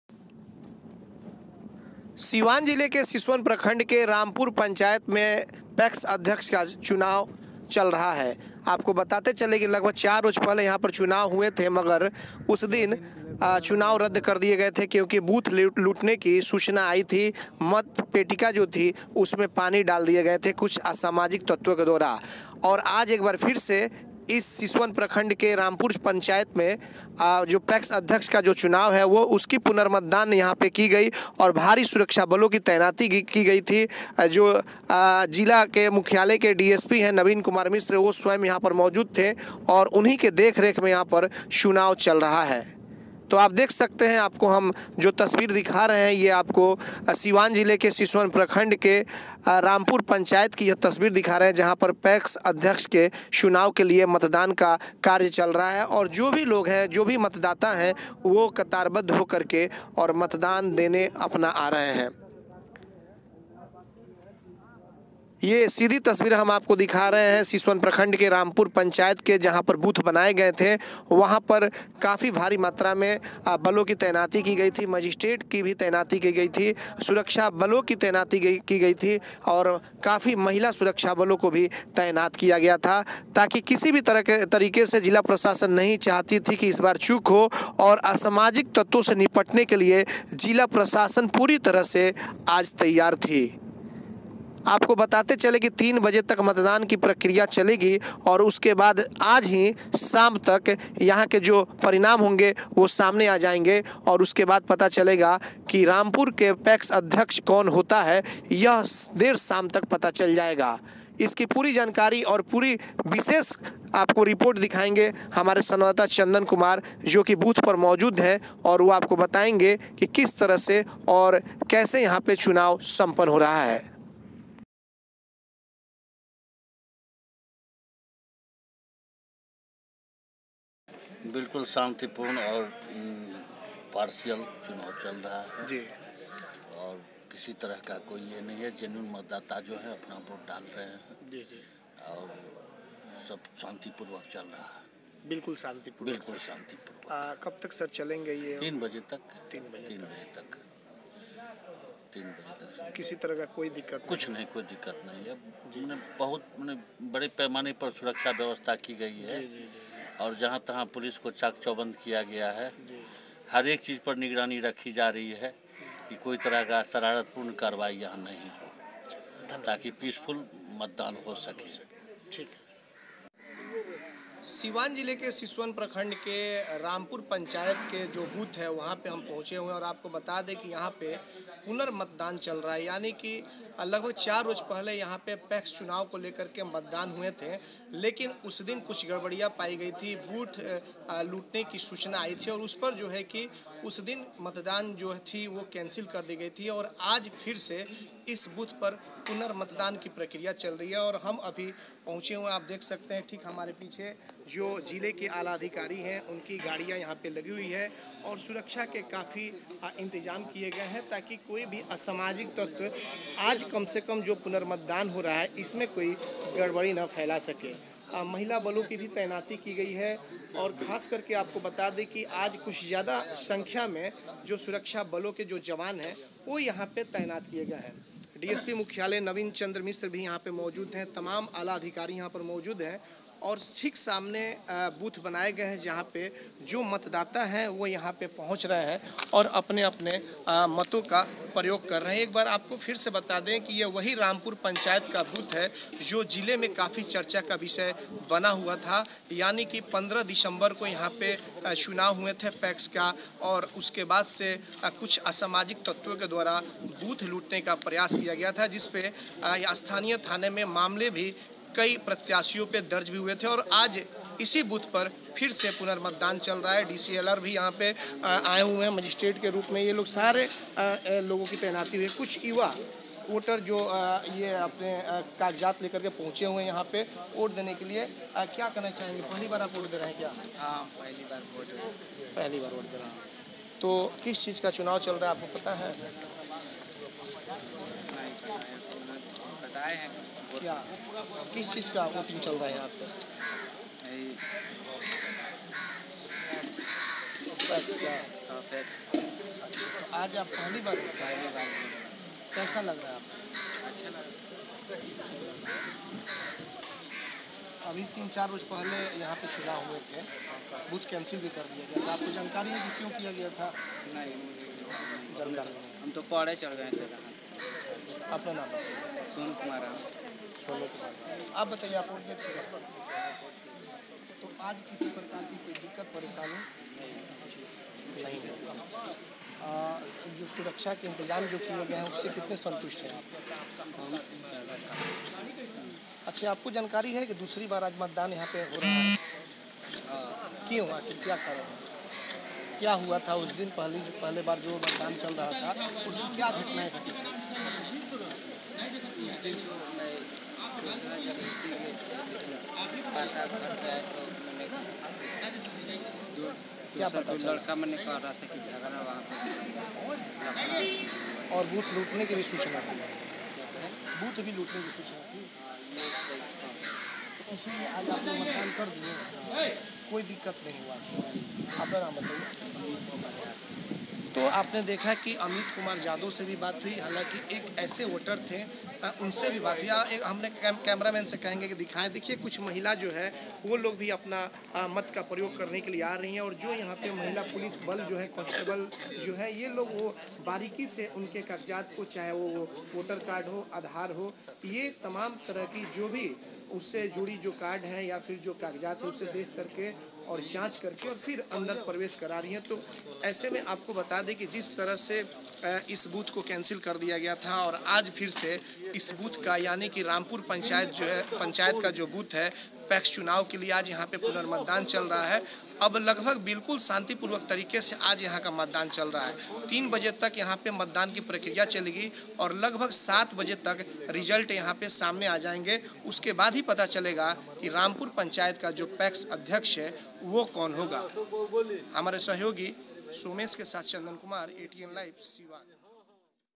siwan# सिसवन प्रखण्ड के रामपुर पैक्स अध्यक्ष का पुनर्मतदान शांतिपूर्ण सम्पन्न-atnlive.in
atnlive.in